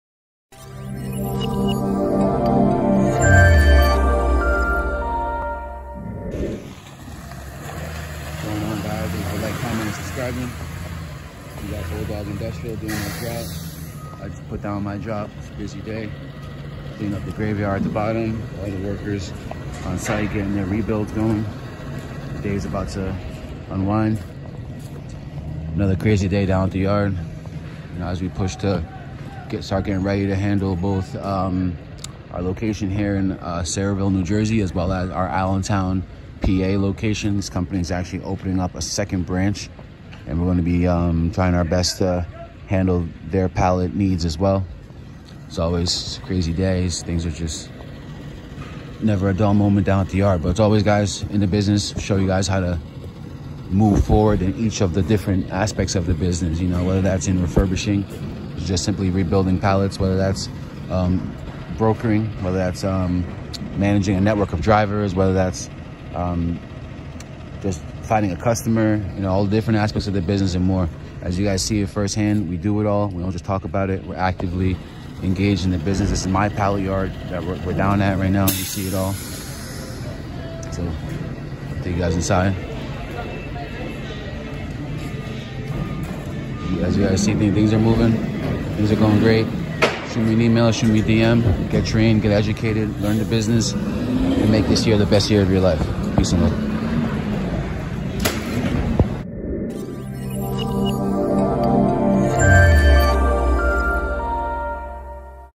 On pace to do 1.2 Million dollars this year
This year we are on pace to sell 1.2 million dollars worth of pallets

I have a course/mentoring program I offer to anyone interested in learning the business.